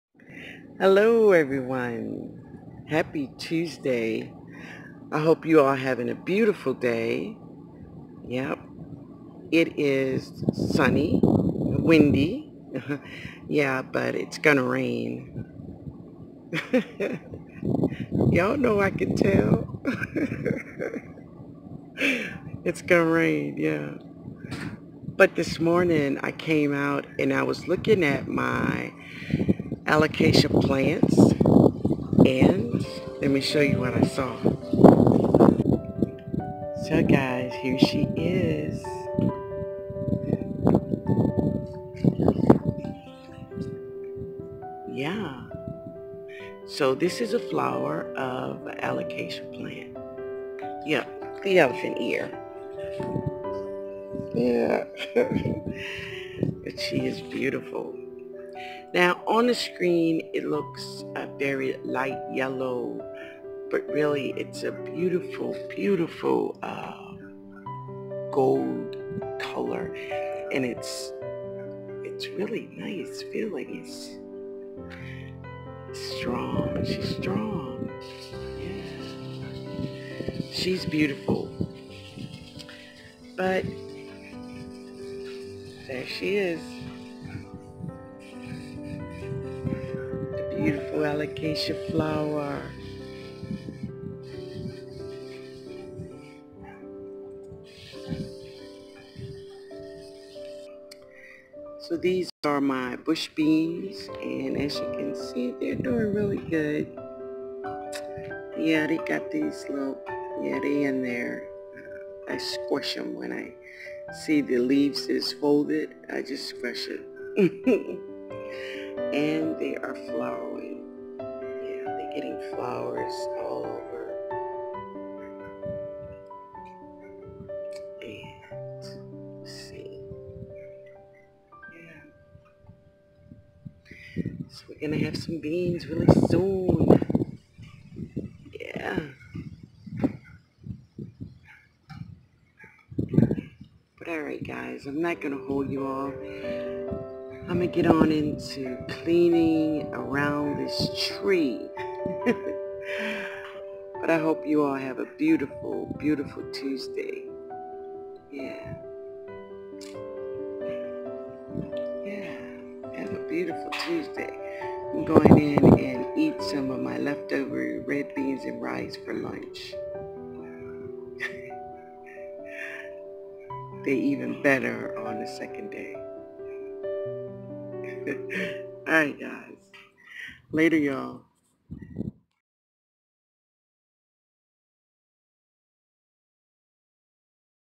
The Alocasia Odora Flower { The Elephant Ear }
Good Morning Everyone In today's video, I missed the BLOOMING of the first Alocasia Odora Flower, But I have many plants in my Garden, So this Morning, She had Bloomed and waiting for ME.